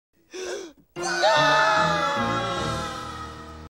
My Reaction That Rooster Teeth Will Be Shutting Down Soon
I Don't Believe It! First, YouTube Editor, Next, Google+, Then, GoAnimate For Schools, Adobe Flash Player, Qubo, Blue Sky Studios, NickRewind, Teletoon And EShop On Nintendo 3DS And Wii U, And Now Rooster Teeth Will Be Shutting Down Soon?! THE HORROR!